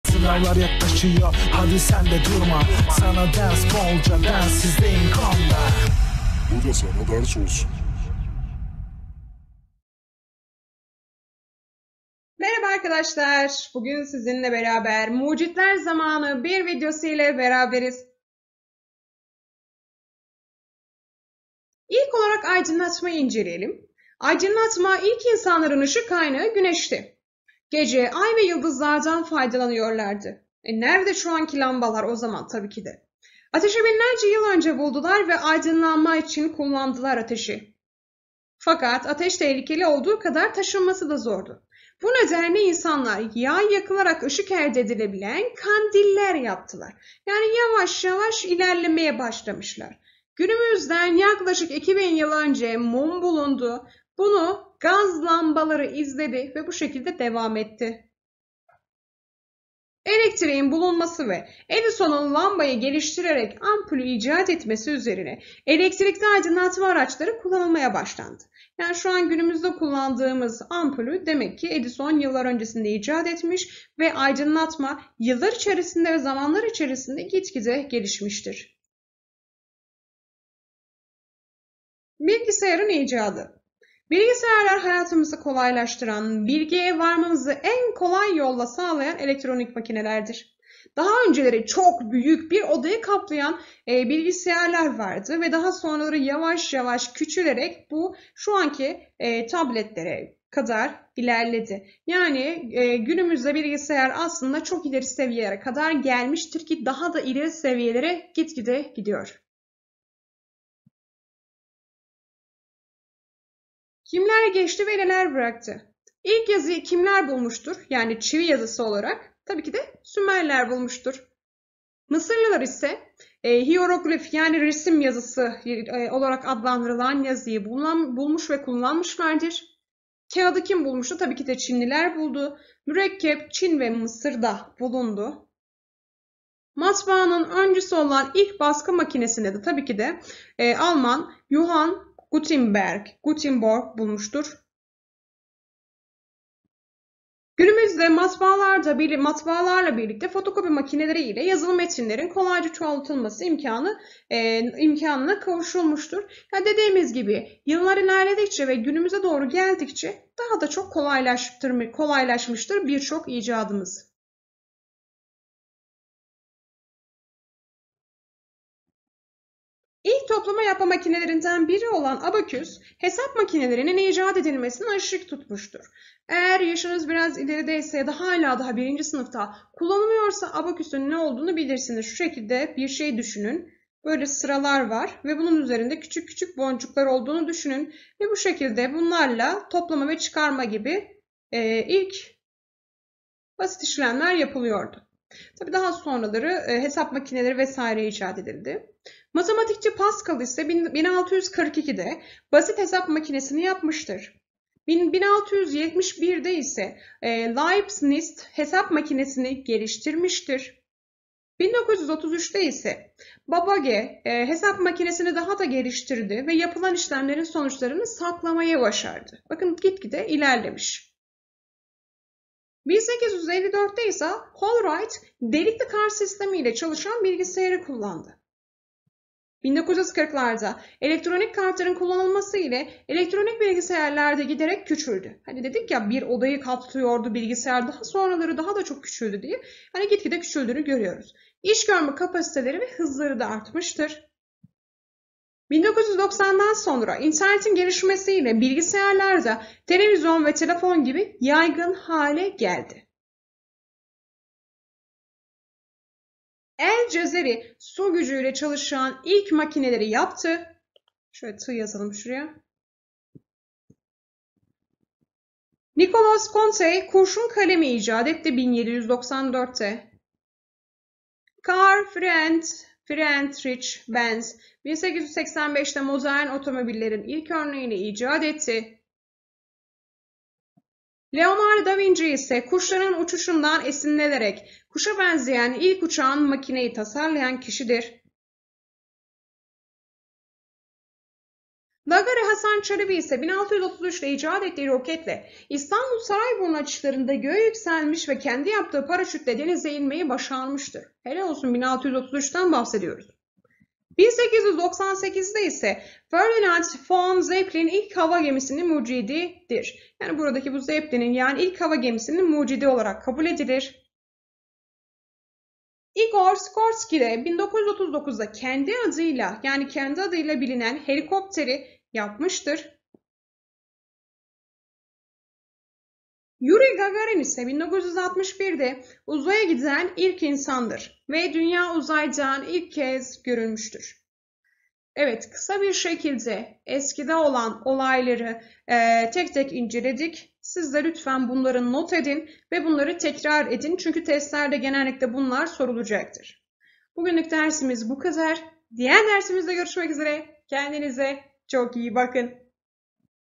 BİLİM - TEKNOLOJİ VE TOPLUM - 2 / MUCİTLER ZAMANI 1
İlkokul 4. Sınıflar için hazırladığımız Sosyal Bilgiler dersinin bu bölümünde Bilim, Teknoloji ve Toplum ünitesinin Mucitler Zamanı alt başlığının 1. bölümü anlatılacaktır. Dersi takip ederek konuyu öğrenebilir ya da bilgilerinizi pekiştirebilirsiniz.İyi seyirler ve başarılar.